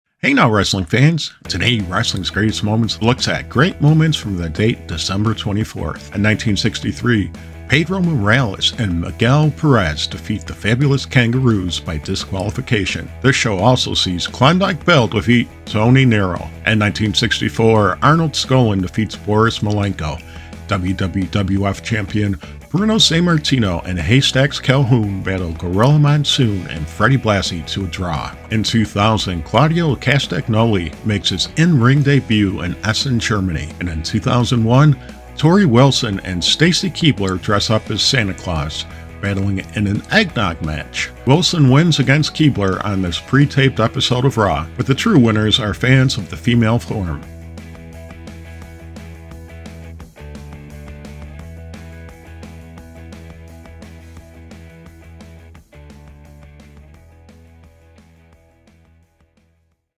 December 24 in Pro Wrestling Volume One
December 24th in Professional Wrestling. A look at some great moments in wrestling from December 24th including Stacy Kiebler and Torrie Wilson's unforgettable Eggnog Match. @wrestlingsgreatestmoments  looks at some of the key matches and events from today.

                                              Works Referenced
“Cards on This Day in History.” The Internet Wrestling Database. Accessed 18 Dec. 2023.

“WWWF—2001 Results.” The History of the WWE. Accessed 19 Dec. 2023.